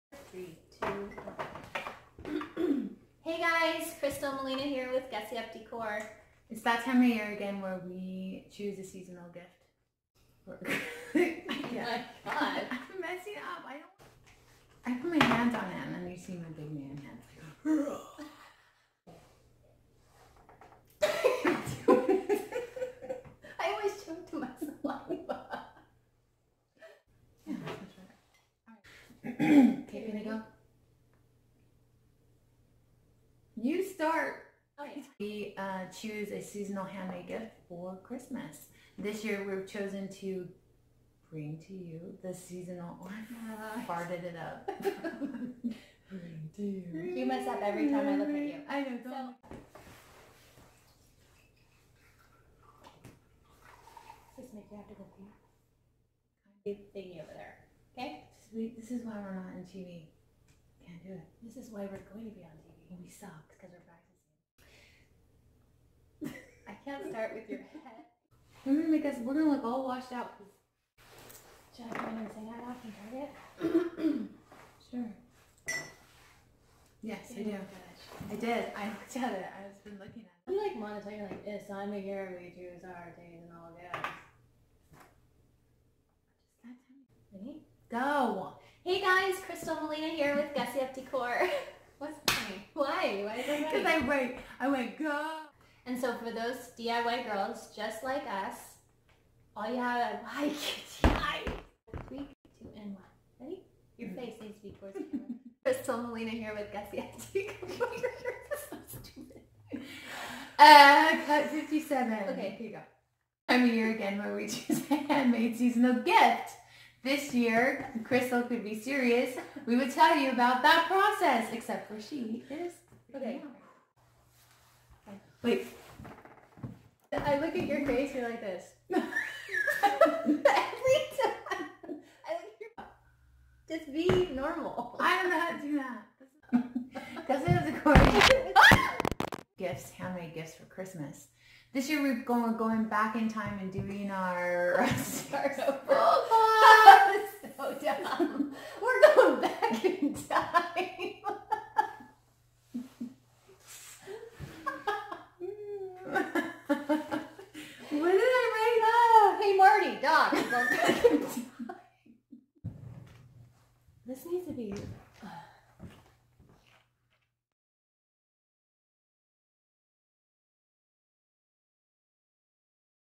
Gussy Up Decor's BLOOPERS
Best out takes/bloopers. This is what happens when Malena and I try to make a video.